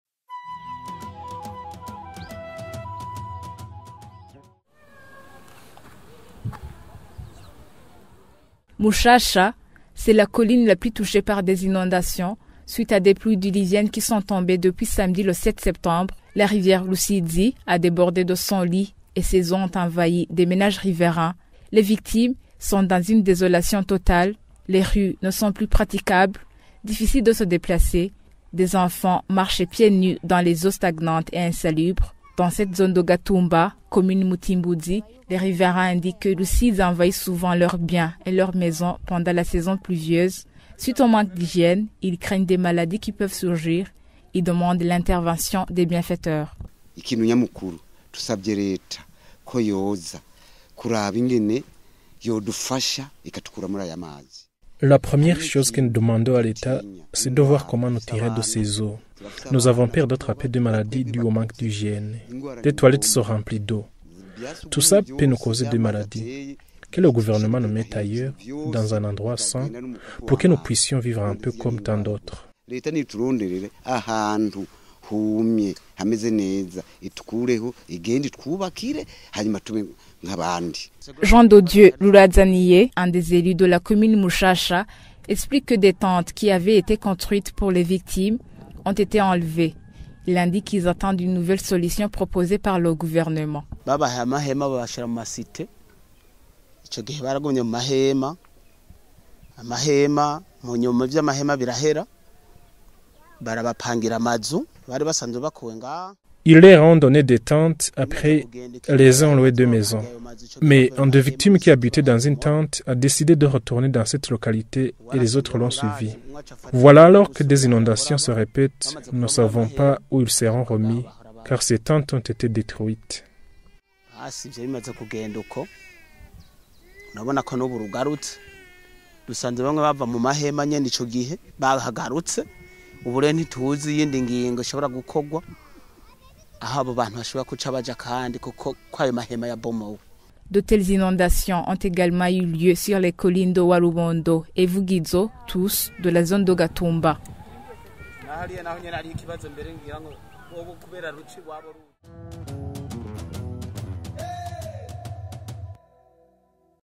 Gatumba: Rusizi ne cesse d’emporter des maisons
Mushasha, c’est la colline la plus touchée par des inondations, suite à des pluies diluviennes qui sont tombées depuis samedi le 7 septembre.